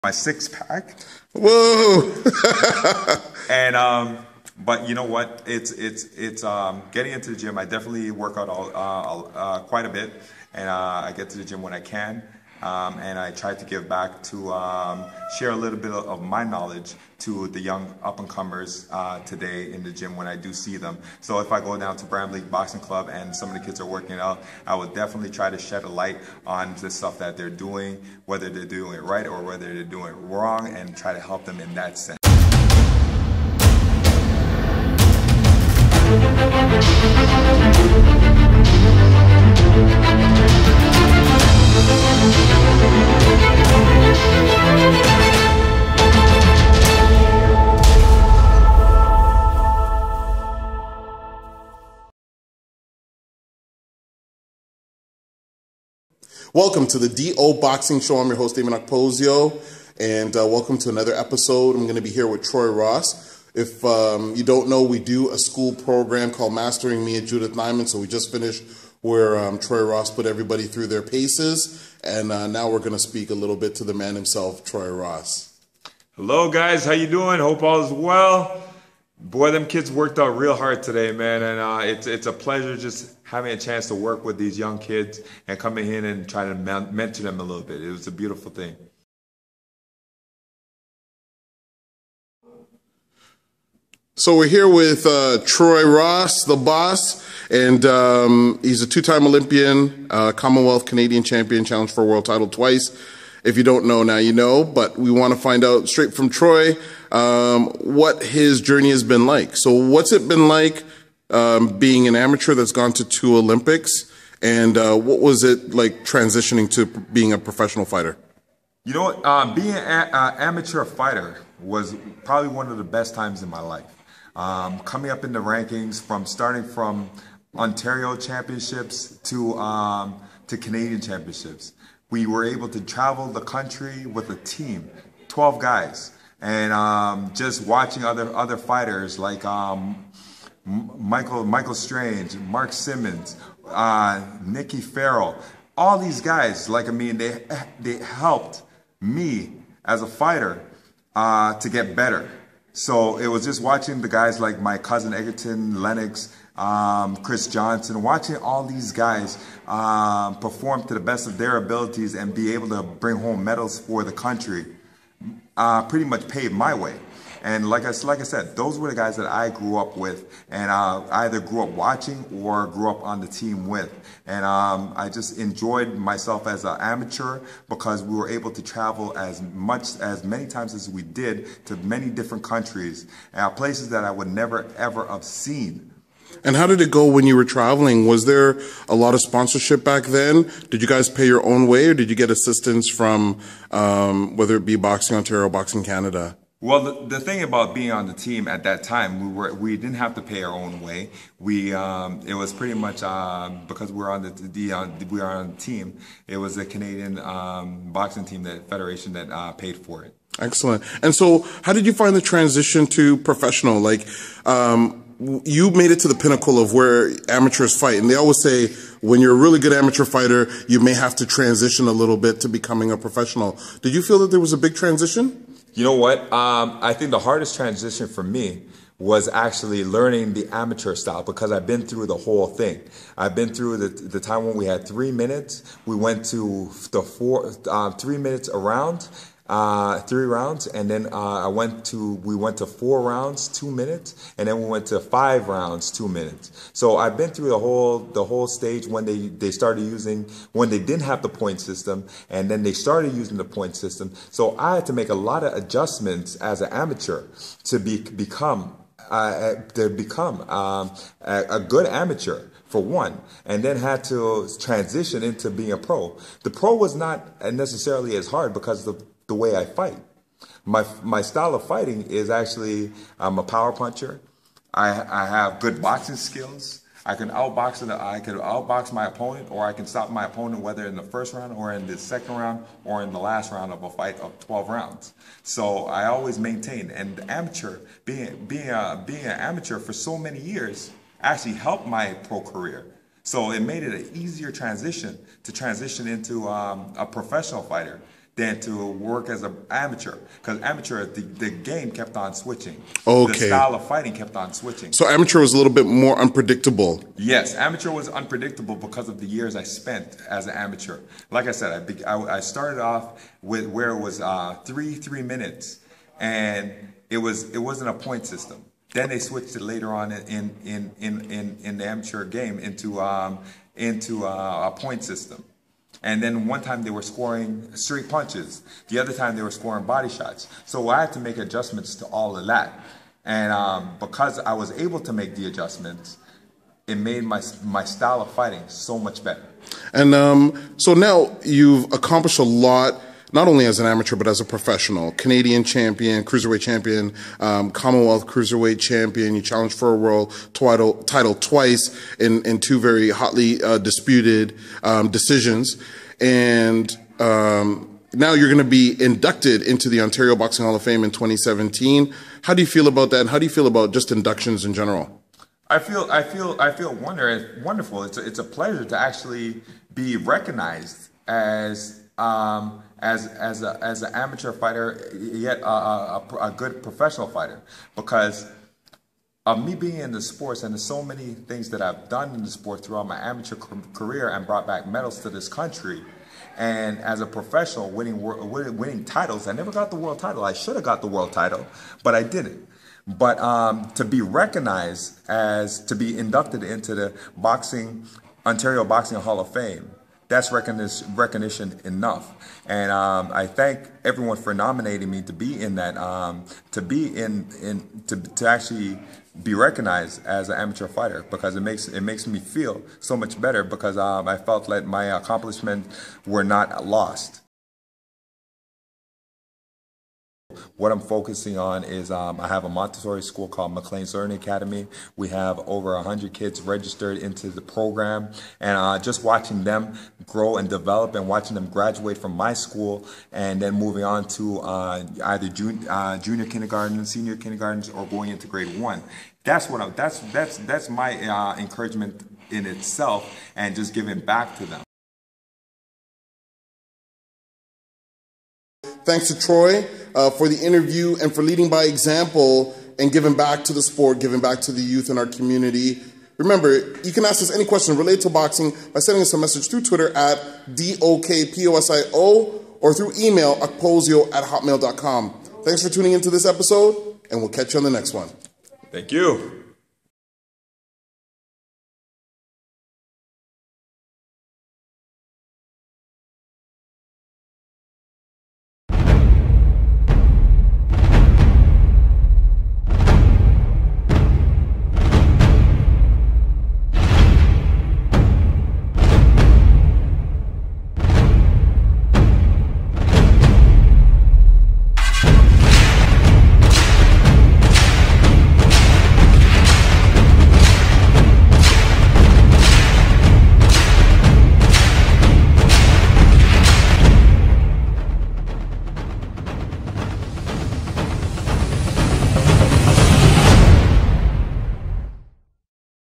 DO Boxing Show – Episode 272 – Troy Ross inducted into Ontario Boxing Hall of Fame
TROY ROSS
Amateur Accomplishments
Amateur record: 93-7
-2000 Olympic Team member
-1999 Canadian Champion
-1998 Canadian Champion
-1998 Gold medal, Pre-Pan American Tournament
-1998 Silver medal, Commonwealth Games
-1997 Canadian Champion
-1997 Silver medal, Francophone Games
-1996 Canadian Champion
-1996 Olympic Team – Quarter finalist (5th)
-1996 Silver medal, Multi Nations Tournament, Liverpool
-1995 Gold medal, Tammer Tournament, Finland
-1994 Gold medal, Copenhagen Box Cup
-1994 Gold medal, Magadan, Russia, Tournament
Professional Accomplishments
Professional record: 25-3 (16 KO’s)
-Ranked as fourth best Cruiserweight in the world by Ring Magazine
-Season 4 Contender Champion
- Commonwealth (British Empire) Cruiserweight Champion
- Canadian Cruiserweight Champion
- June 5, 2010, challenged Steve Cunningham for the IBF (International Boxing Federation) Cruiserweight Championship
- September 15, 2012, challenged Yoan Pablo Hernandez for the IBF (International Boxing Federation) Cruiserweight Championship
Troy Ross will be inducted into the Ontario Boxing Hall of Fame on Saturday, May 27, 2017.